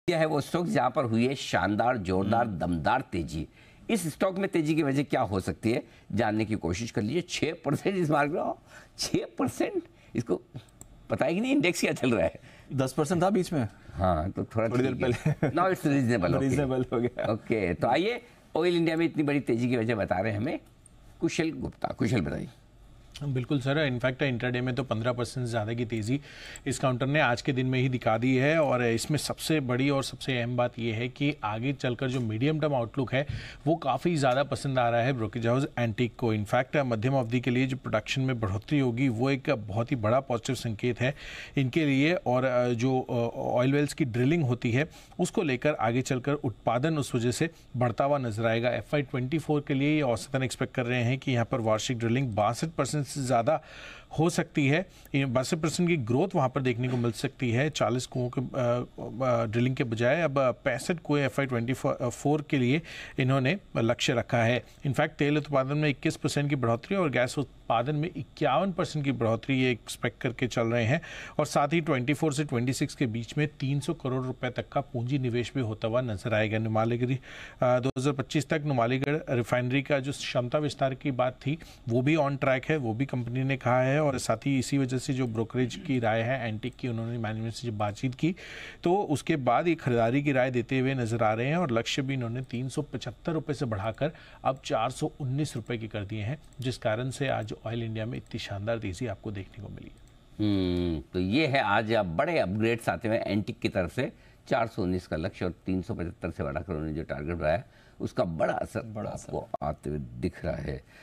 Bull Run in Oil India Stock, the Explosive Momentum of Oil India Stock!
Bull Run in Oil India Stock: एक्शन में OilIndia का शेयर, Share में कहां से आई जोरदार तेजी? आज क्यों 15% से ज्यादा उछला ऑयल इंडिया, देखिए पूरी डिटेल्स इस वीडियो में